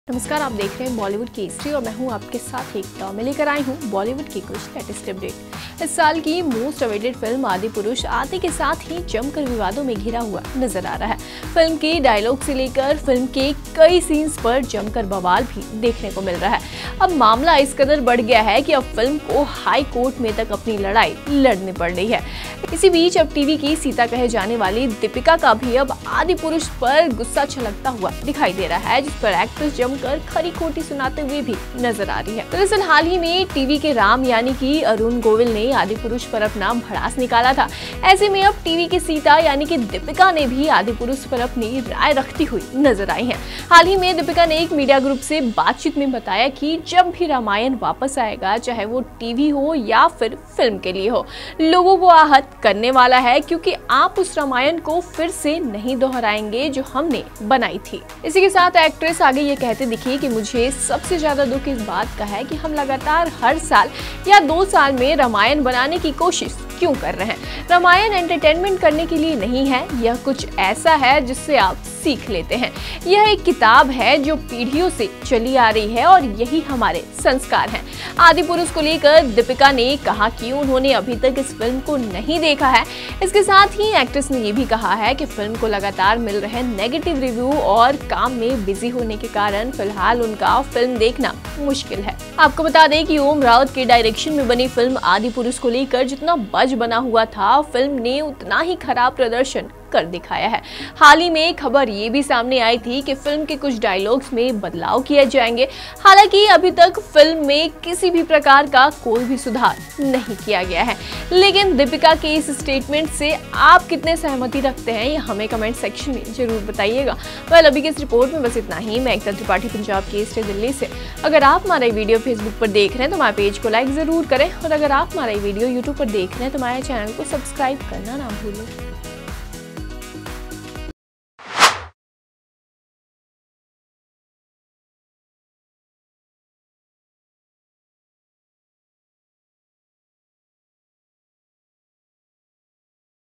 Adipurush पर रामायण की सीता Dipika Chikhlia ने सुनाई खरी-खोटी, कहा- 'रामायण एंटरटेनमेंट नहीं'
Recently TV's Ram i.e. Arun Govil had vented his anger on Adipurush, now TV's Sita Deepika Chikhaliya has also been seen giving her opinion on Adipurush. Recently, Deepika told a media group that- "Whenever Ramayana comes back, whether it is for TV or film, it is going to hurt people, because you will not repeat the Ramayana that we made. "